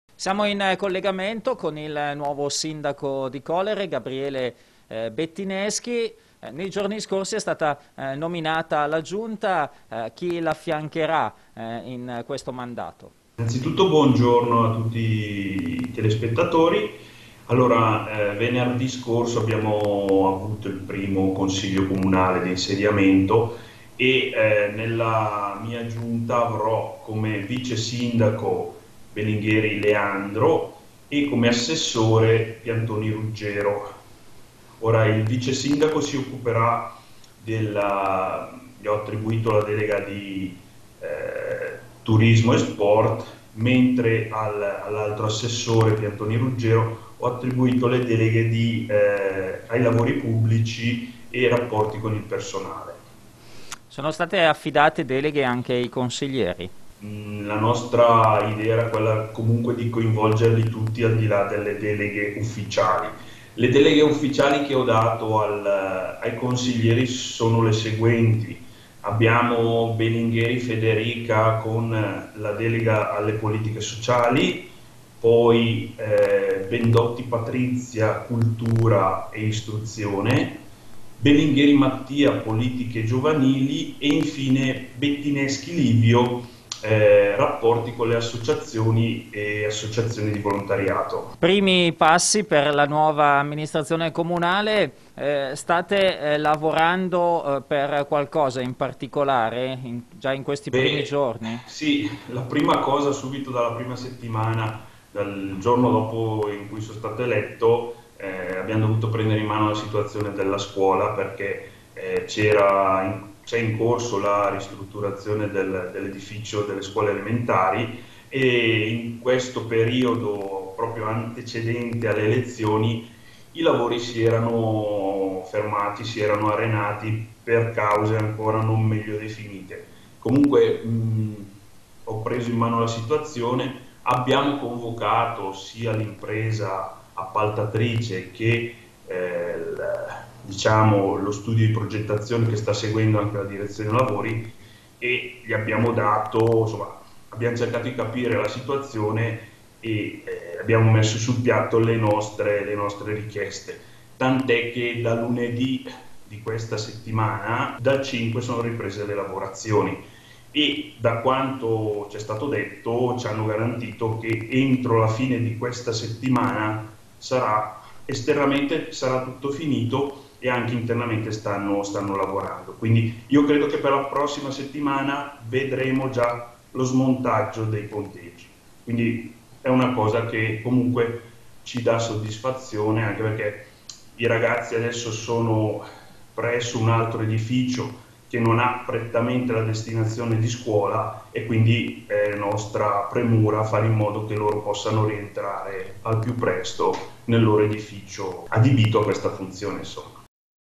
Colere, i primi passi dell'amministrazione di Gabriele Bettineschi
Primi passi e primi problemi da risolvere per la nuova amministrazione comunale guidata dal sindaco Gabriele Bettineschi.